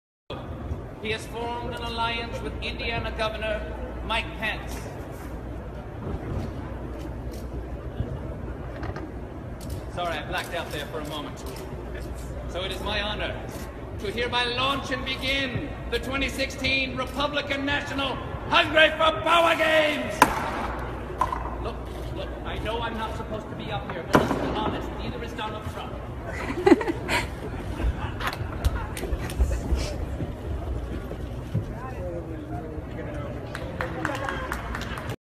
Stephen Colbert takes over the RNC stage 'Hunger Games' Prank [Sound Clear]
Stephen Colbert crashes RNC stage for 'Hunger Games' prank

A video posted online shows "Late Show" host Stephen Colbert taking over the microphone on stage in Cleveland in a "Hunger Games" themed prank at the site of the Republican National Convention, which begins Monday.